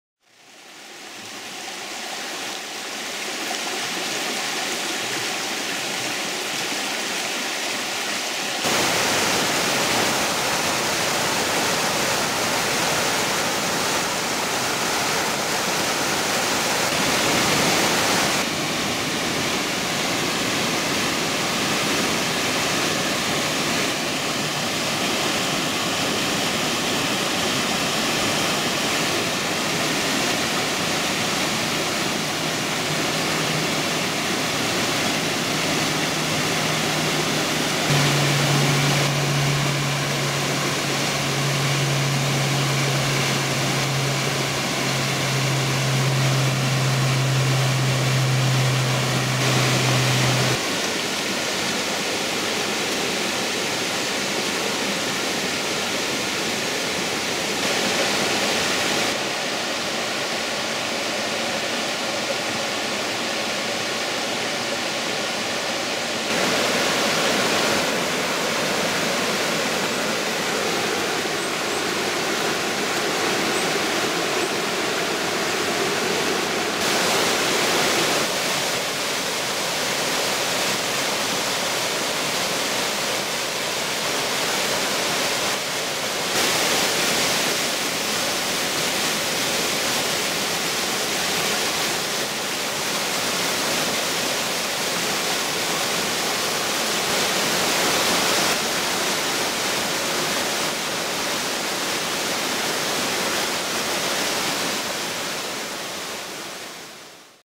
Upper and Lower Waterfalls on Gragg Prong, Wilson Creek area, NC
Upper and Lower Waterfalls on Gragg Prong, Wilson Creek area, Pisgah National Forest, NC. Shot 10/29/13